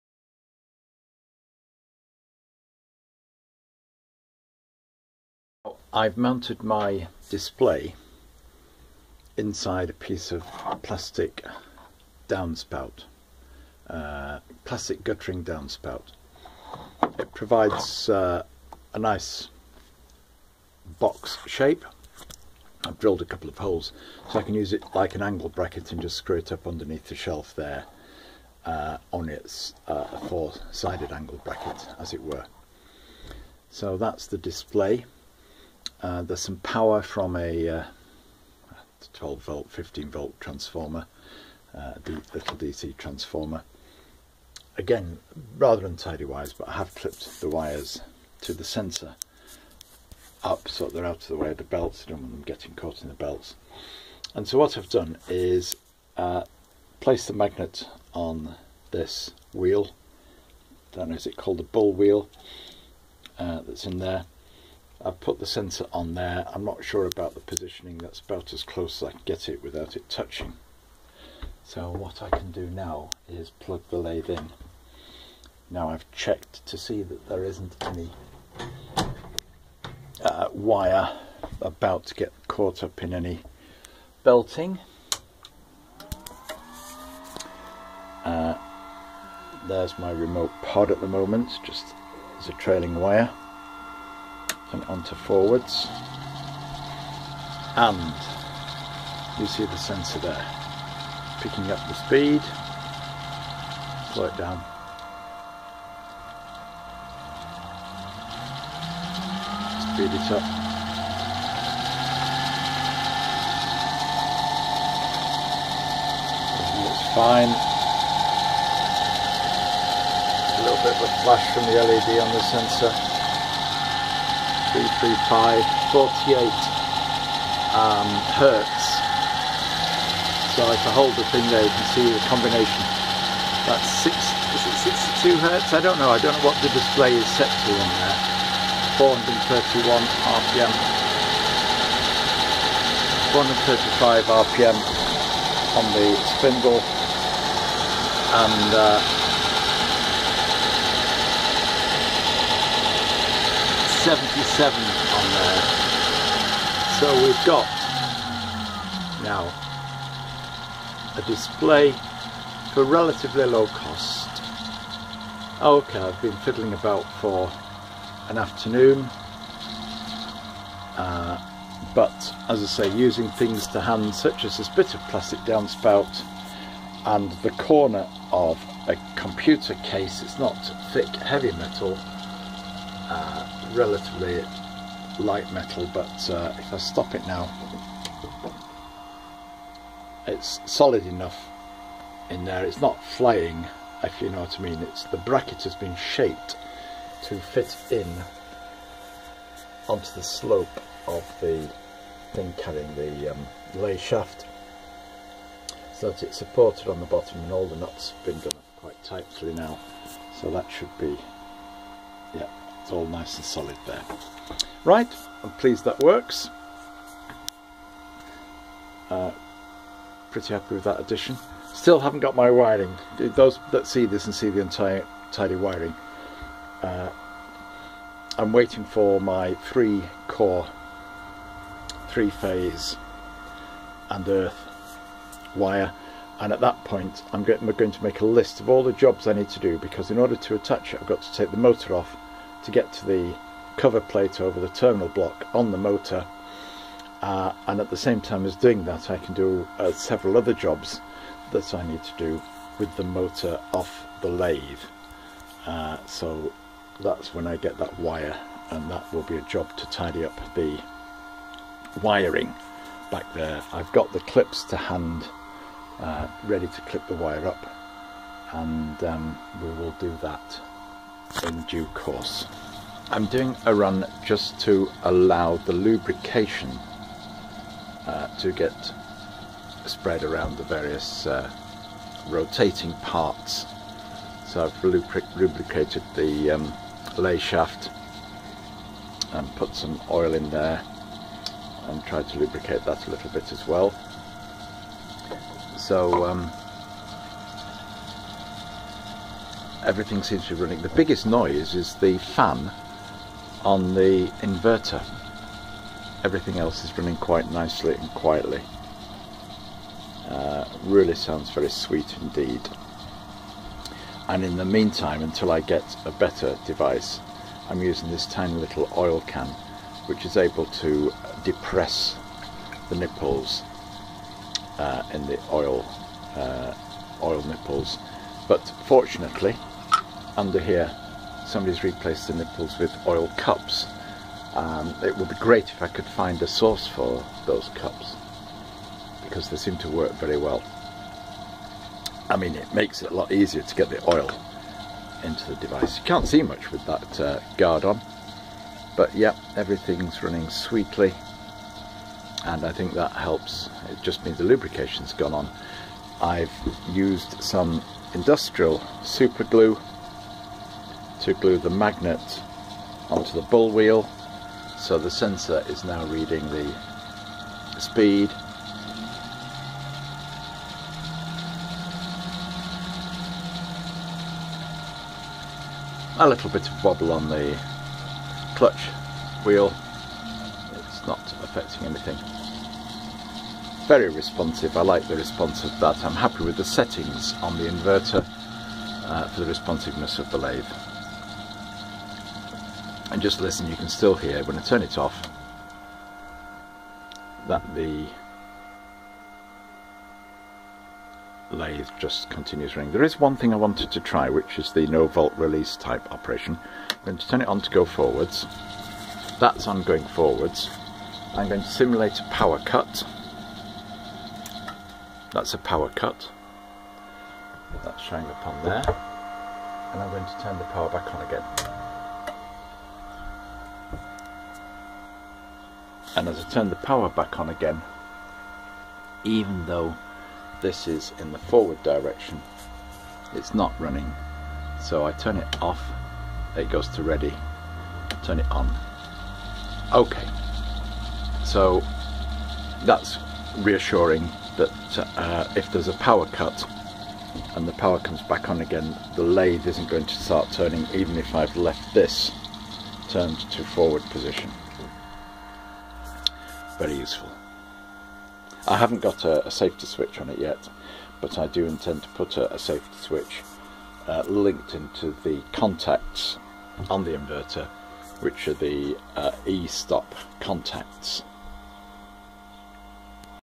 Altivar 11 and tachometer fitted to Myford lathe
The Telemecanique or Schneider Electric Altivar 11 paired with a three phase 1.5 HP motor to drive a Myford Super 7 lathe.